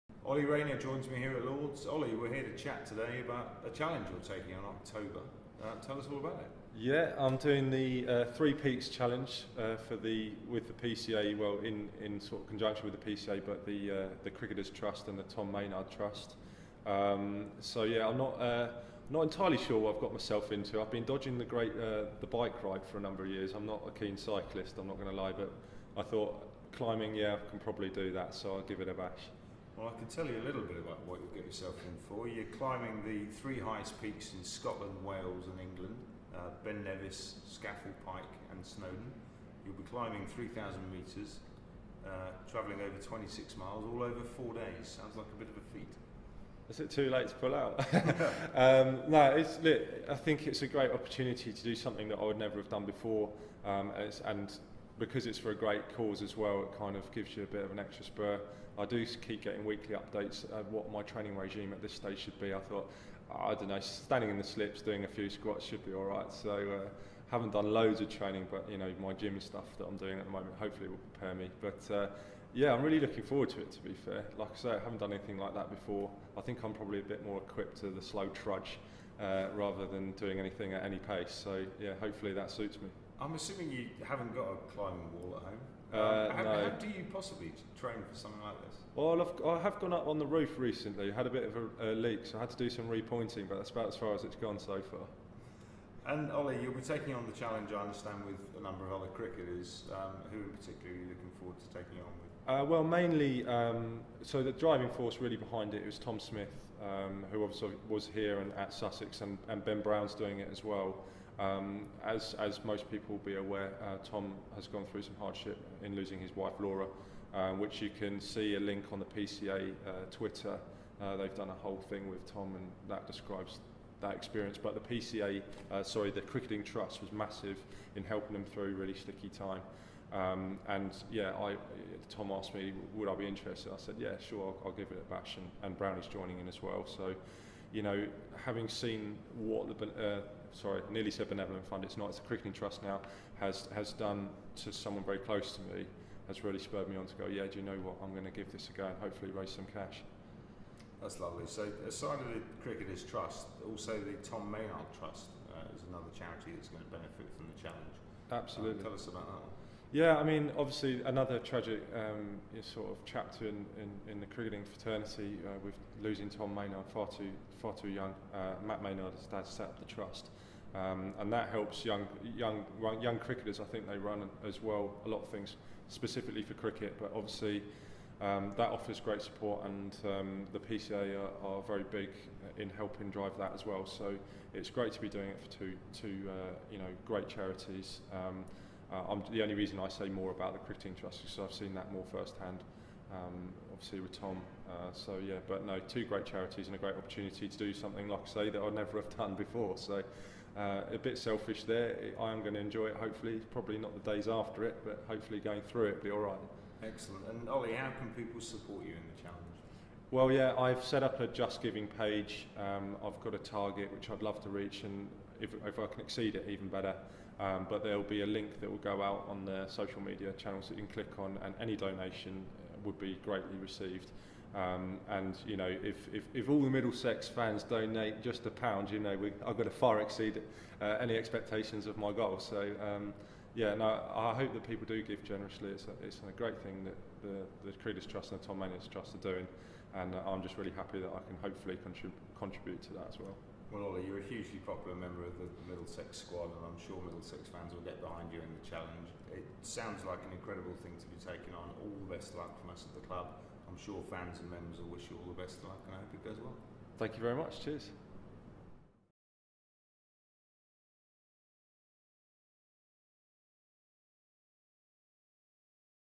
OLLIE RAYNER TAKES ON THE THREE PEAKS CHALLENGE
Middlesex Cricketer Ollie Rayner talks to us through his motivation for taking on the gruelling Three Peaks Challenge for the PCA Cricketers Trust and the Tom Maynard Trust.